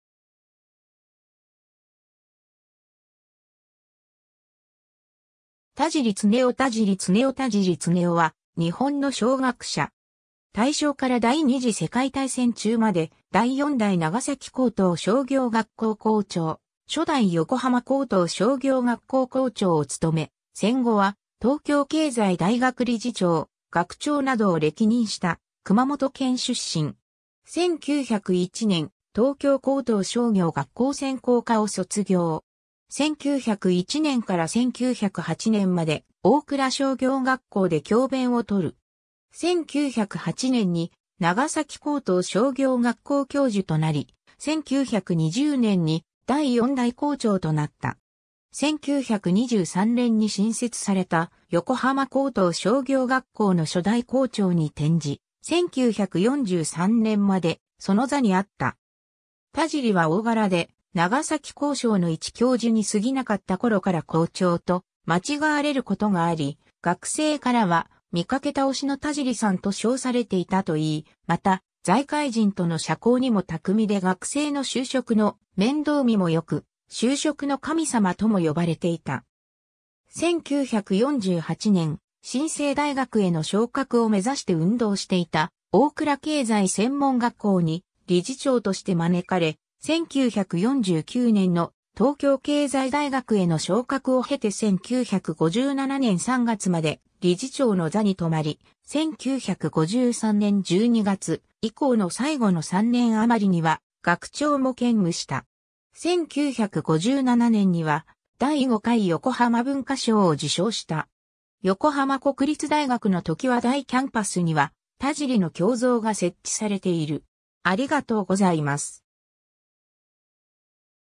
田尻常雄
田尻常雄 田尻常雄 田尻 常雄（たじり つねお、1877年 - 1957年4月18日）は、日本の商学者。
大正から第二次世界大戦中まで、第4代長崎高等商業学校（現長崎大学）校長、初代横浜高等商業学校（現横浜国立大学）校長を務め、戦後は東京経済大学理事長、学長などを歴任した、 熊本県出身。
 1901年、東京高等商業学校（現一橋大学）専攻科を卒業。
1901年から1908年まで大倉商業学校で教鞭を執る。
 1908年に長崎高等商業学校（現長崎大学）教授となり、1920年に第4代校長となった。
1923年に新設された横浜高等商業学校（現横浜国立大学）の初代校長に転じ、1943年までその座にあった。
 田尻は大柄で、長崎高商の一教授に過ぎなかった頃から校長と間違われることがあり、学生からは「見かけ倒しの田尻さん」と称されていたといい、また、財界人との社交にも巧みで学生の就職の面倒見もよく「就職の神様」とも呼ばれていた。
 1948年、新制大学への昇格を目指して運動していた大倉経済専門学校に理事長として招かれ、1949年の東京経済大学への昇格を経て1957年3月まで理事長の座に留まり、1953年12月以降の最後の3年余りには、学長も兼務した。
 1957年には第5回（昭和31年度）横浜文化賞を受賞した。
 横浜国立大学の常盤台キャンパスには、田尻の胸像が設置されている。